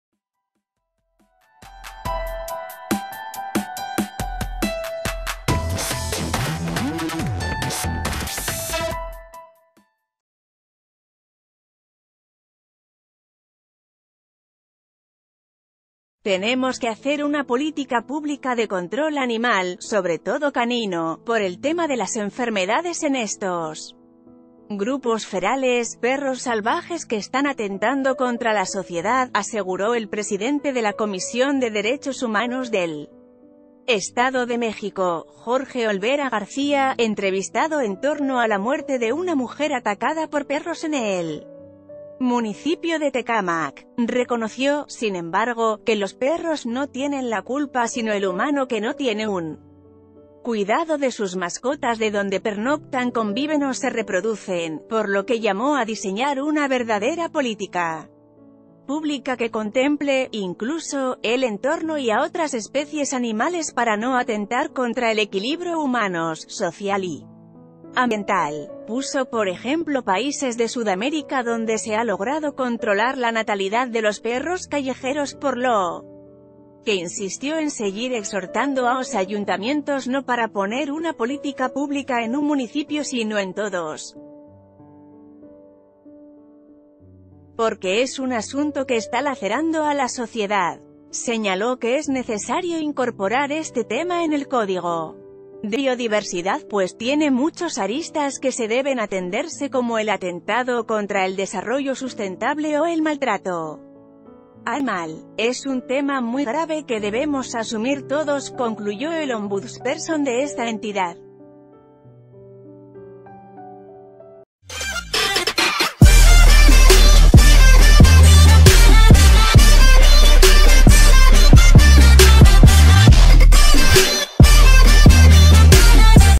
Urgen política de control animal en Edomex
“Tenemos que hacer una política pública de control animal, sobre todo canino, por el tema de las enfermedades en estos grupos ferales, perros salvajes que están atentando contra la sociedad”, aseguró el presidente de la Comisión de Derechos Humanos del Estado de México, Jorge Olvera García, entrevis...